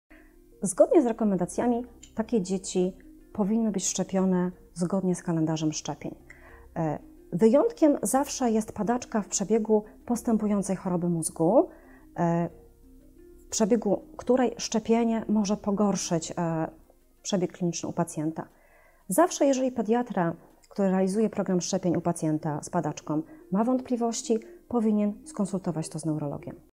Czy można szczepić dziecko z padaczką?
Czy można szczepić dziecko z padaczką? Odpowiada Dr n. med. Magdalena Anna Janeczko  specjalista pediatrii i genetyki klinicznej